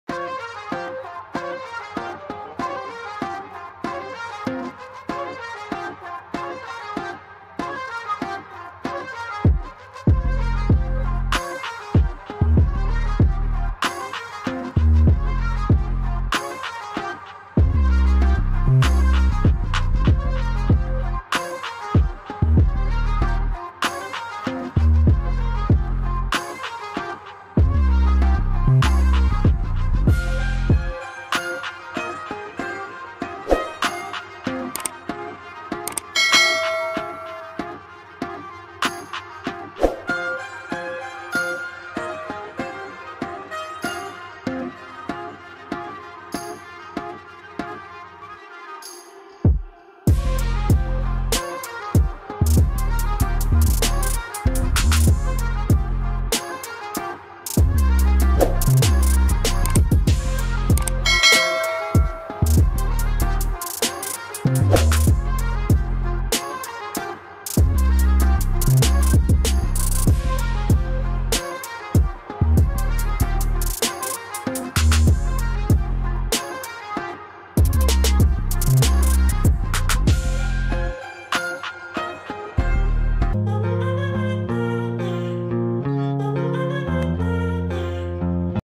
Create a Gradient and Overlapping Text Effect with Reflection in Canva | Mazrify
In this video I'II show you how to create a gradient and overlapping text effect with reflection in canva, Elevate your projects and captivate your audience with this easy-to-follow tutorial. Watch now and transform your Canva creations!